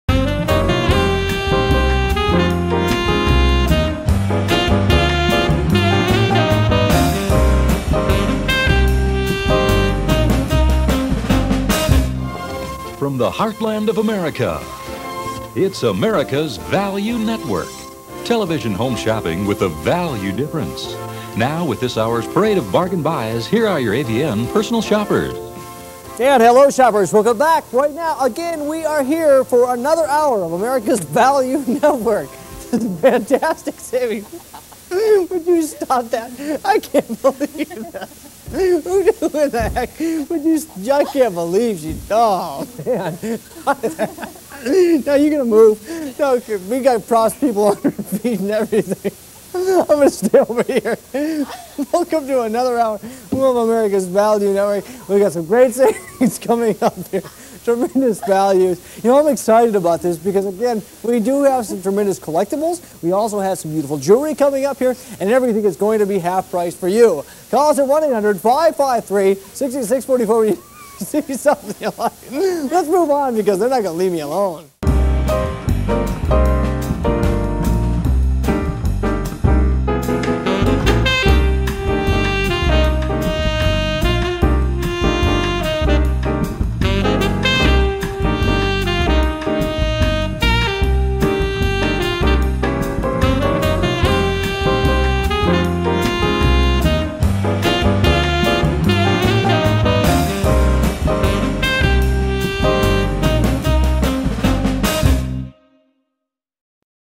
Crotch Grabbed on National Television!
One of the most infamous moments in home shopping history. National Television Host John Cremeans last day at AVN - America's Value Network before heading to CVN - The Cable Value Network. The crew decides to play a trick and unsuspecting to the host a product specialist has snuck under the host desk. What happens next is priceless!